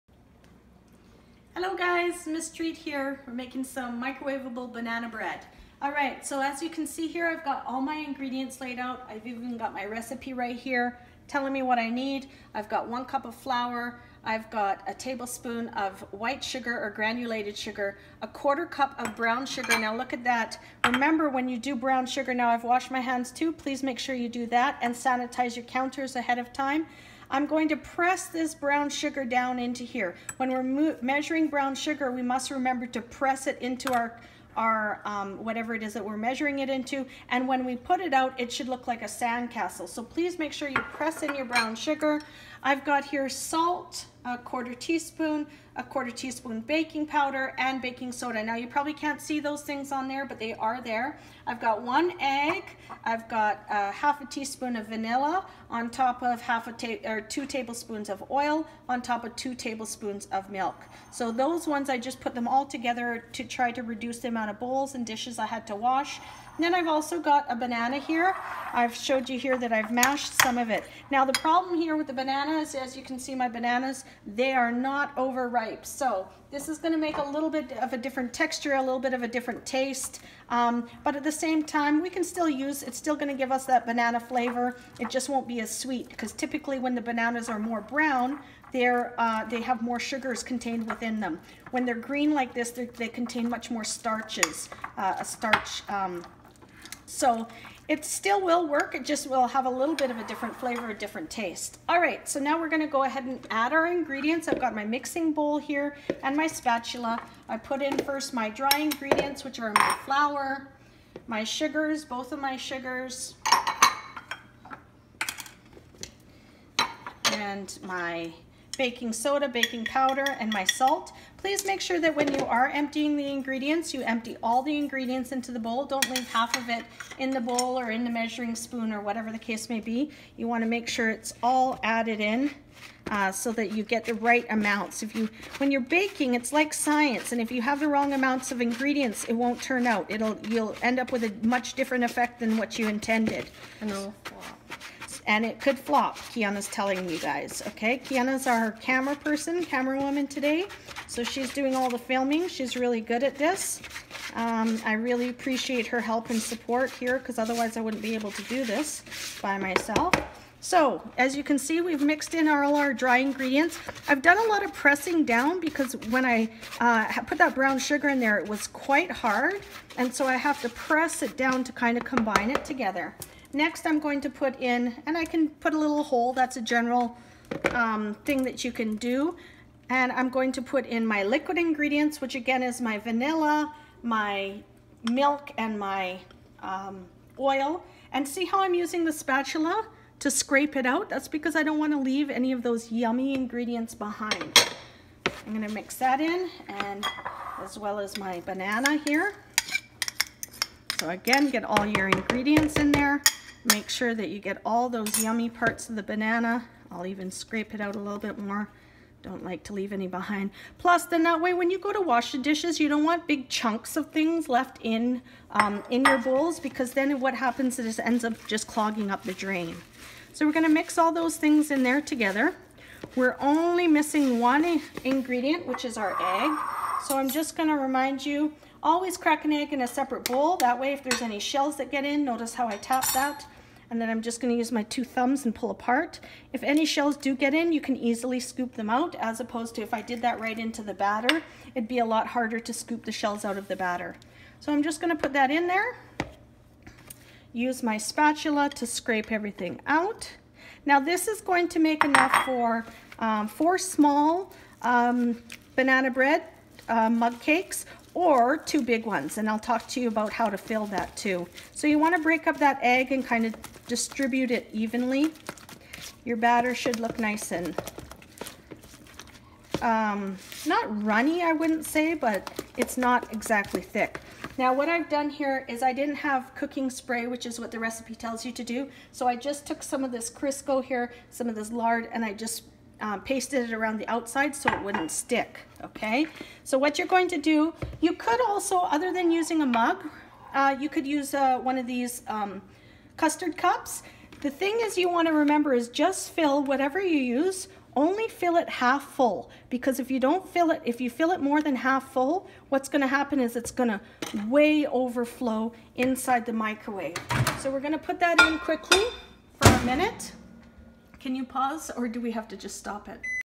Microwaved Banana Bread Part 1
Let's make banana bread in a mug in the microwave!!  Follow the recipe provided and make yourself a fabulous dessert.  Let me know how it goes!!